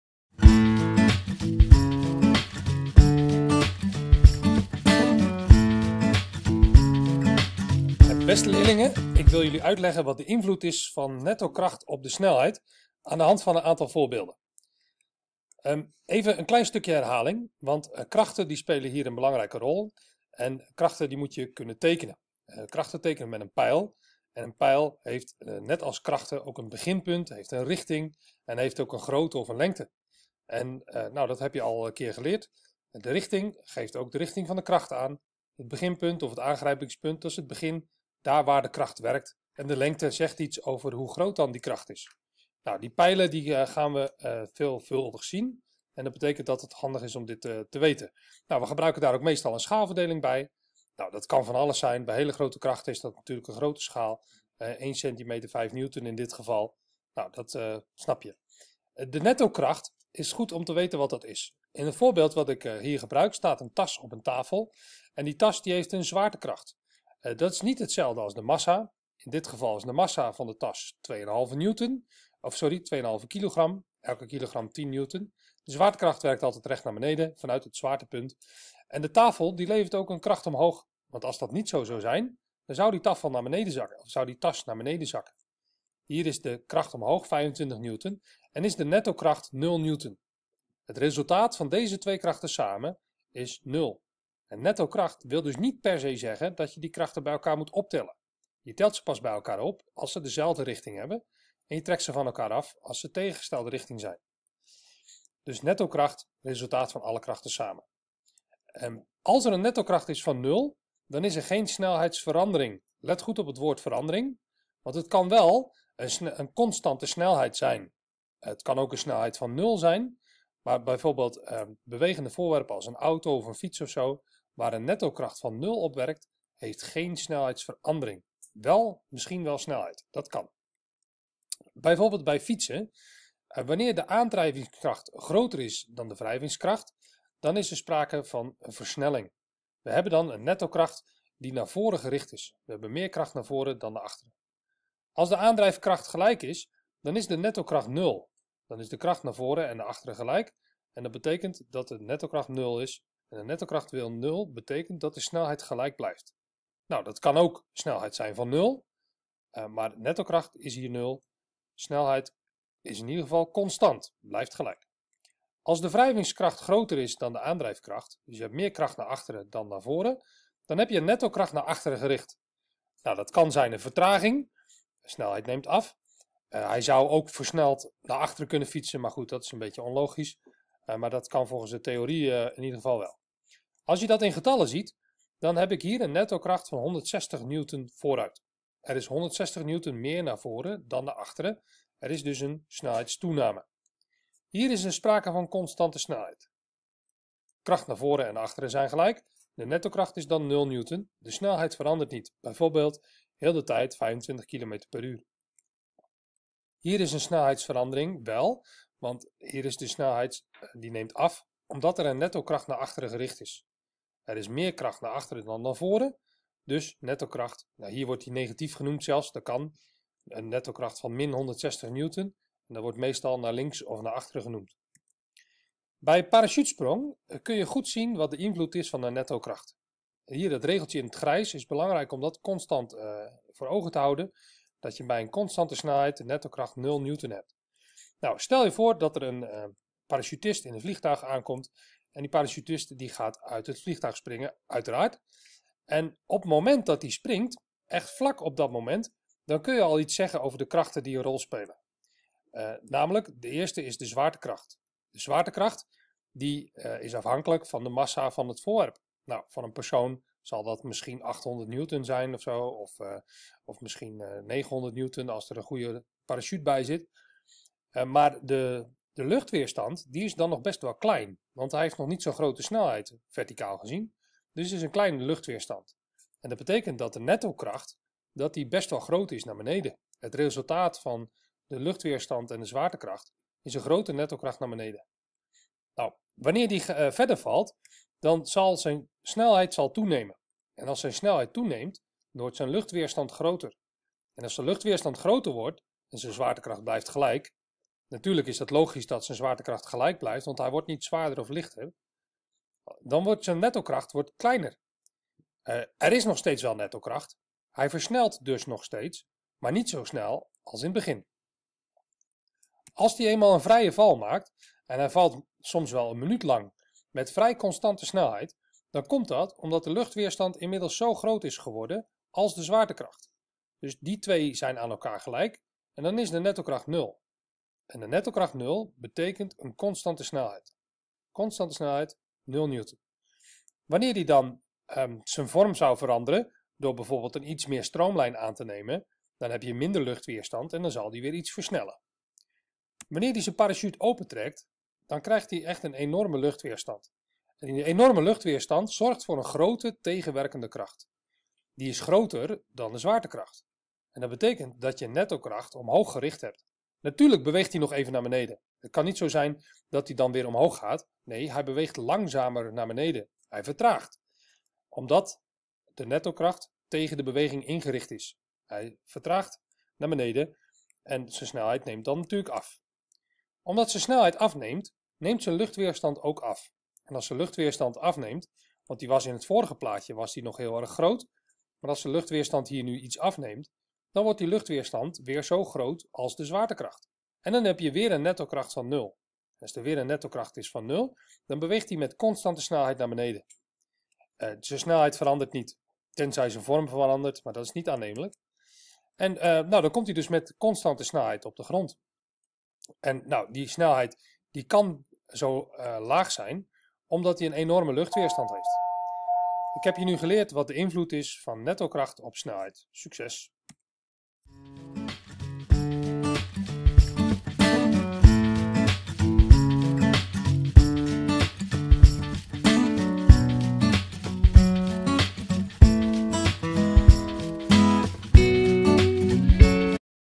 H3-V3 na - HST 4 - netto kracht en snelheid
Uitleg over de gevolgen van snelheid bij een bepaalde netto-kracht.